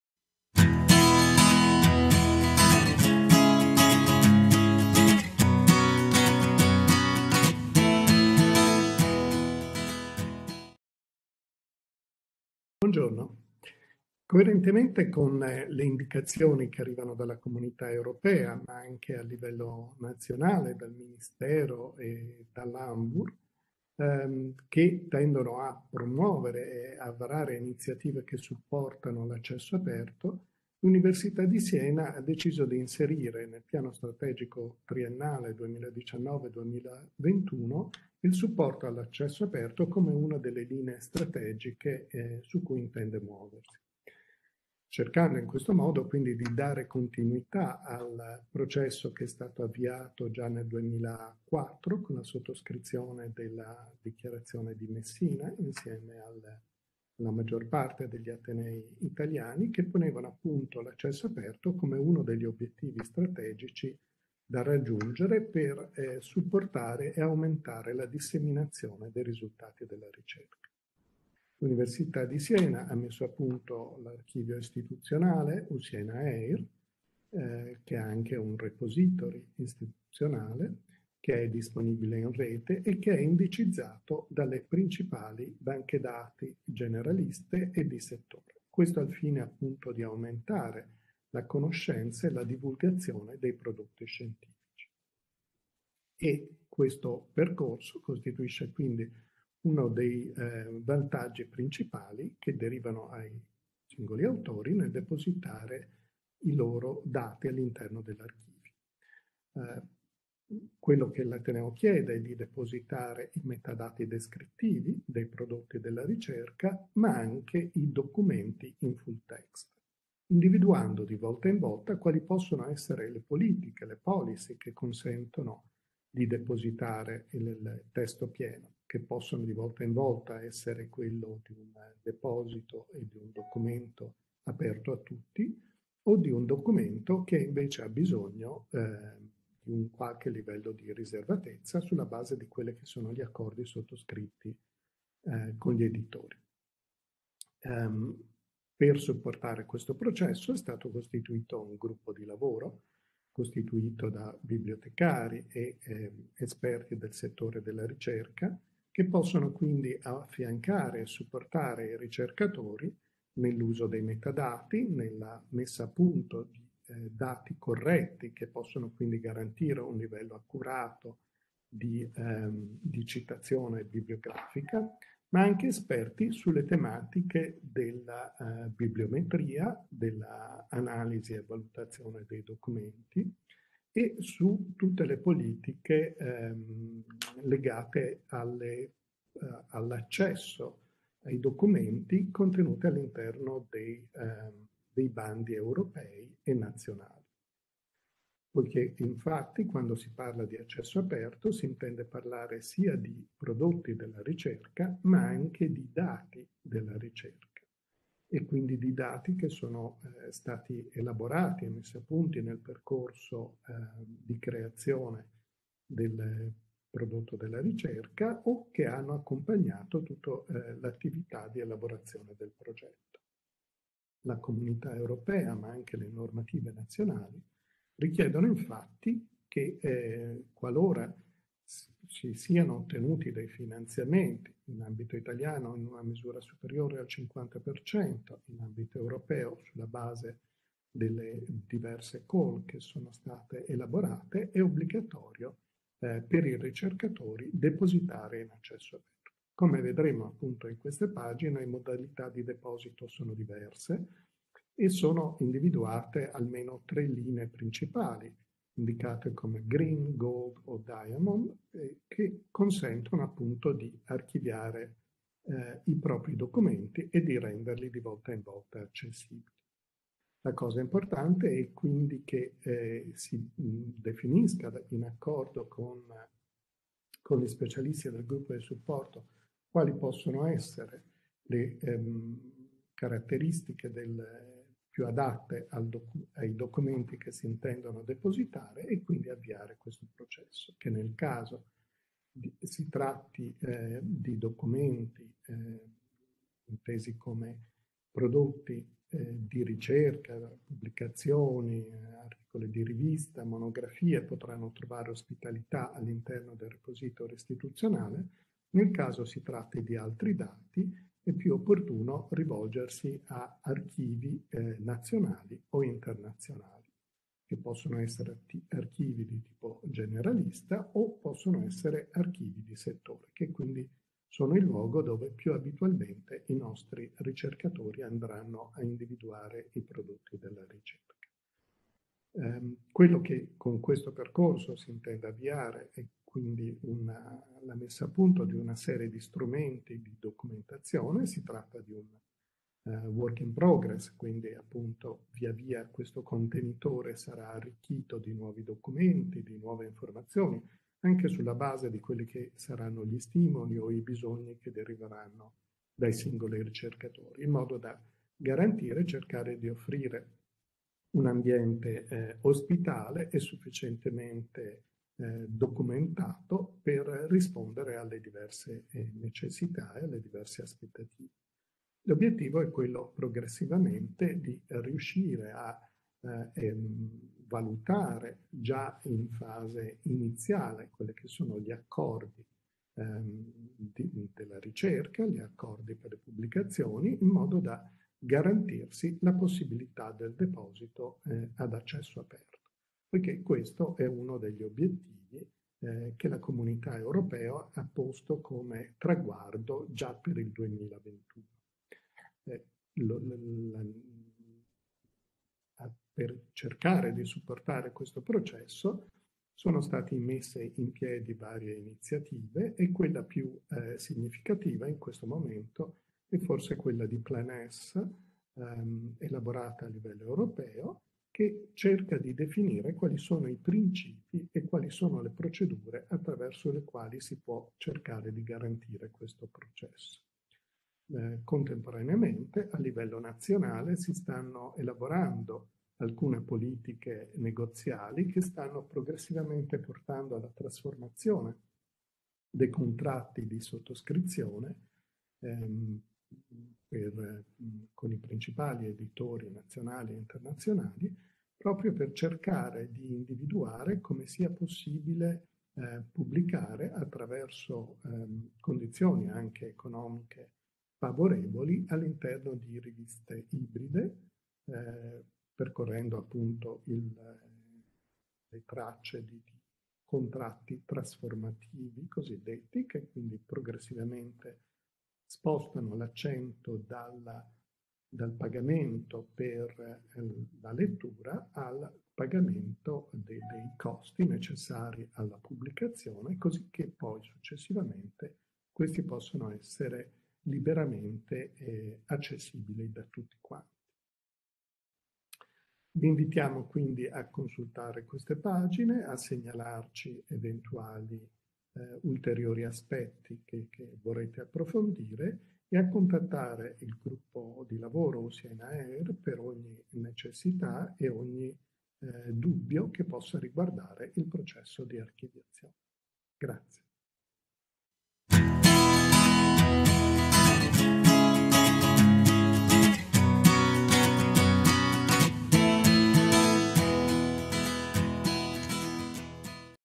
Le politiche di Ateneo sull'Open Access - OA@unisi
In questo intervento, il dott. Guido Badalamenti, Responsabile dell'Area ricerca, biblioteche, internazionalizzazione e terza missione dell’Università degli Studi di Siena, illustra le politiche di supporto all’accesso aperto adottate dall’Ateneo, coerentemente con le indicazioni della Comunità europea, del Ministero e dall’ANVUR, e in continuità con la sottoscrizione della Dichiarazione di Messina nel 2004.